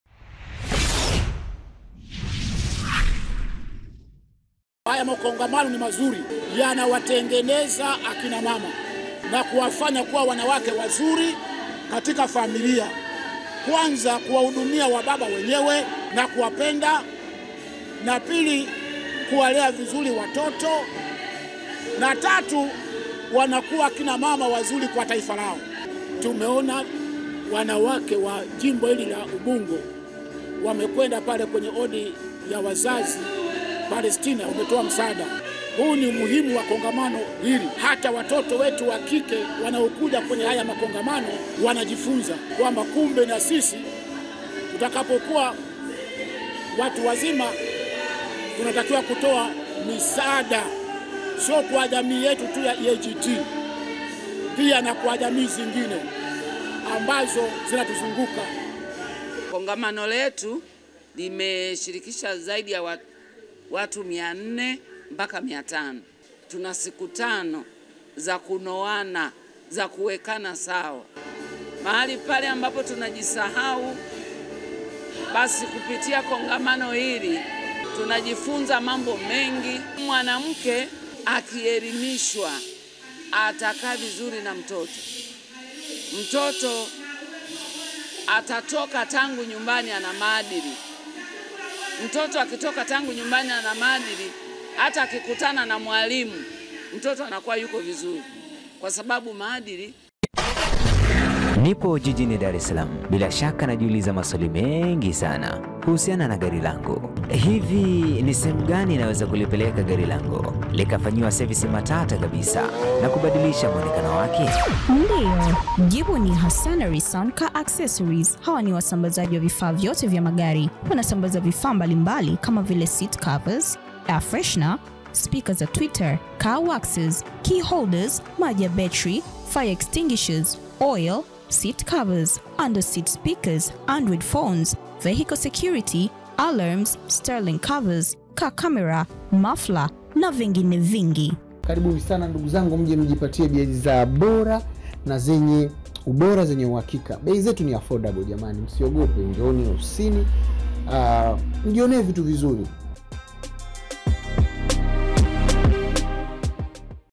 WANAWAKE KANISA la EAGT WAJIFUNGIA SIKU 5 KUKUMBUSHANA MAADILI.
WANAWAKE KANISA la EAGT UBUNGO WAJIFUNGIA SIKU 5 KUKUMBUSHANA MAADILI.

Wanawake zaidi ya 450 wa kanisa la EAGT Jimbo la Ubungo wamekusanyika kwa muda wa siku tano kwenye Kongamano maalumu la kupeana mafunzo ya namna bora ya malezi kwenye familia na jamii kwa ujumla.

Kongamano hilo lilianza rasmi  18 machi hadi 22 machi 2024 lilifanyika katika kanisa la EAGT Sinza Jimbo la Ubungo.